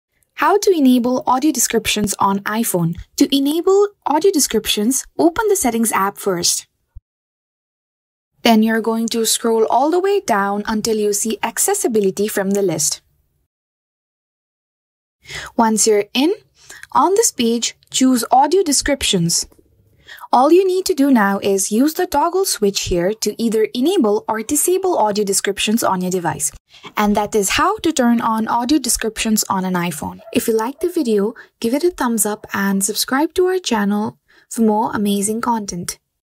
How to Enable Audio Descriptions on iPhone or iPad (iOS)
Step into the world of enhanced accessibility with your iPhone in 2024! This detailed tutorial is designed to guide you through the process of enabling audio descriptions on your iPhone. Perfect for those seeking assistance with visual impairments or anyone who wants to experience their media in a more descriptive, auditory way, this video is a key resource for making your iPhone experience more inclusive.

In this tutorial, we'll cover:
- Navigating the latest iOS settings to find and activate audio description features.
- Understanding how audio descriptions work and their benefits for various users.
- Customizing audio description settings for optimal personal usage.
- Exploring compatible apps and content that support audio descriptions.

Whether you're new to iOS or an experienced user looking to explore accessibility features, this video provides easy-to-follow steps and insightful tips. If you've ever wondered, 'How do I enable audio descriptions on my iPhone?' - this is your go-to guide.